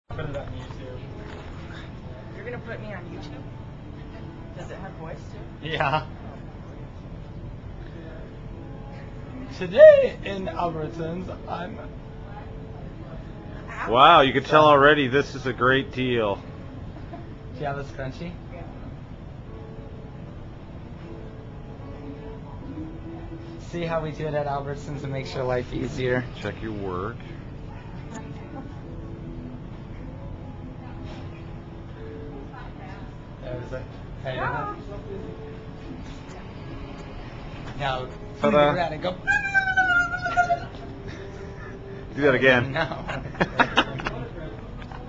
Hair braiding now at Albertson's
Clint braids Melanie's hair at Albertson's.